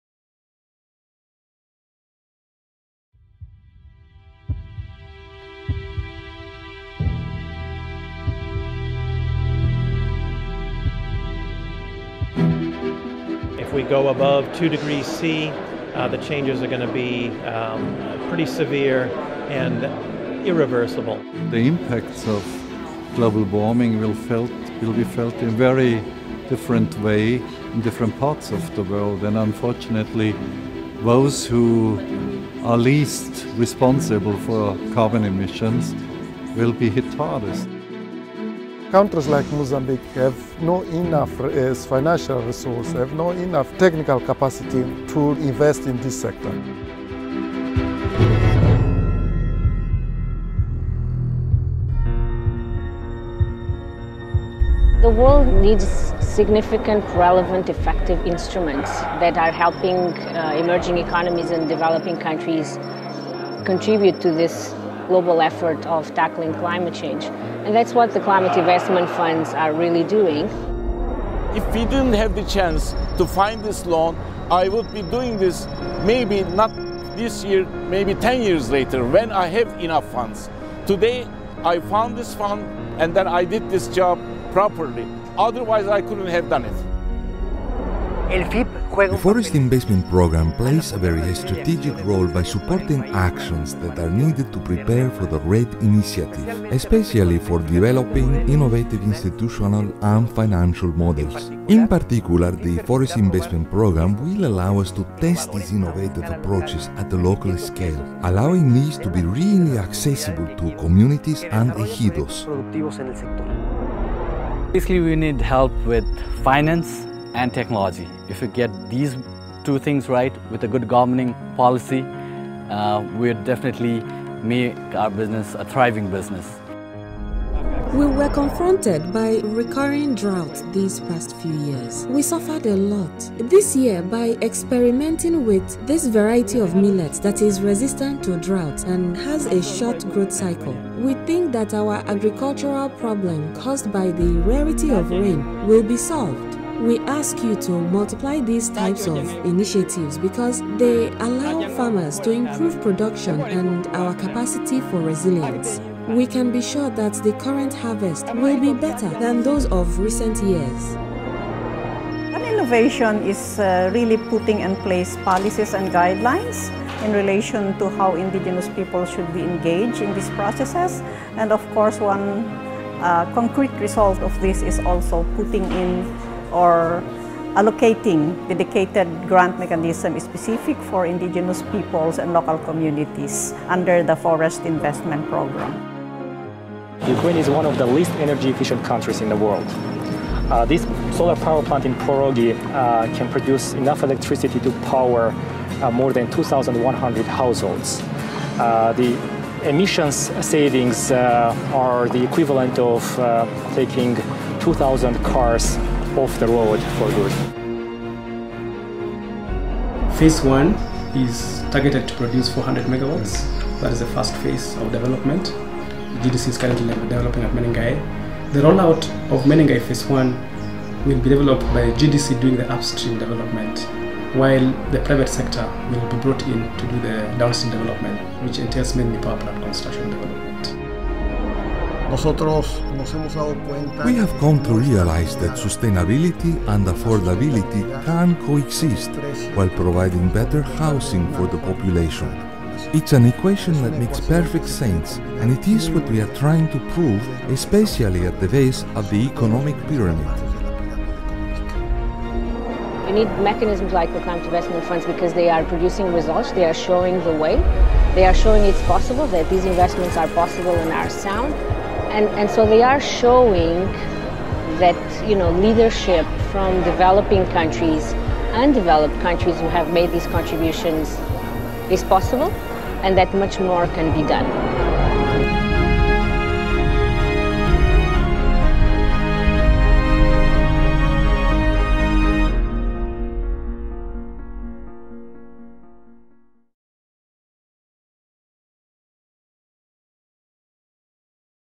صناديق الاستثمار المناخية: تحقيق نتائج على نطاق واسع والتمكين من عملية التحوّل
تحقق صناديق الاستثمار المناخية نتائج على نطاق واسع وتعمل على التمكين من عملية التحوّل. وقد تم عرض هذا الفيديو في دوّر الانعقاد الأول لاتفاقية الأمم المتحدة الإطارية المعنية بتغير المناخ في 2015  في بون بألمانيا في 9 يونيو/حزيران 2015 أثناء فعاليات جانبية في إطار آفاق الدروس المستفادة من صناديق الاستثمار المناخية